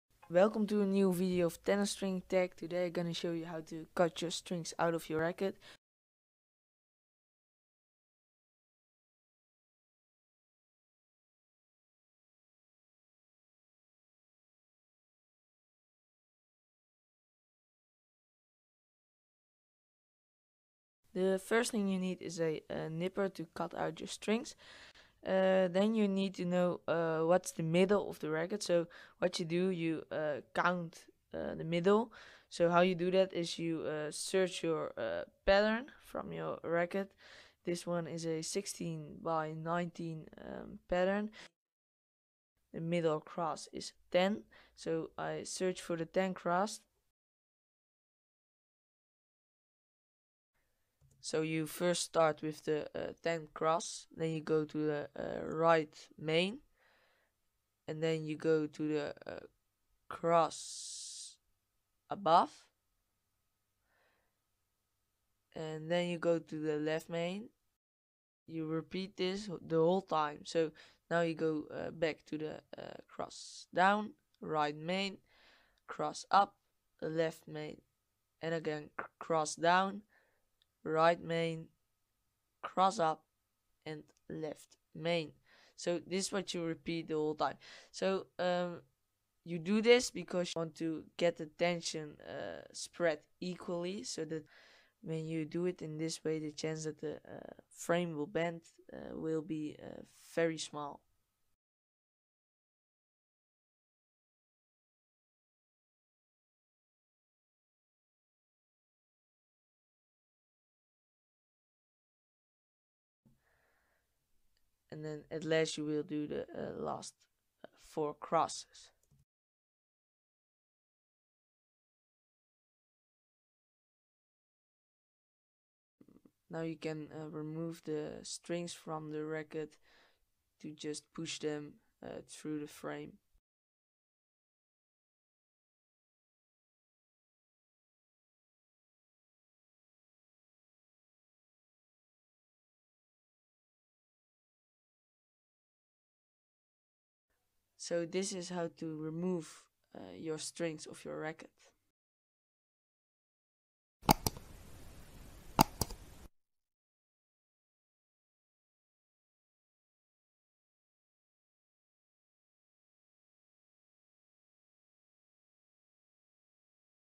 How to CUT strings OUT of a TENNISRACKET?!
In this video I show you how to cut strings out of a tennisracket.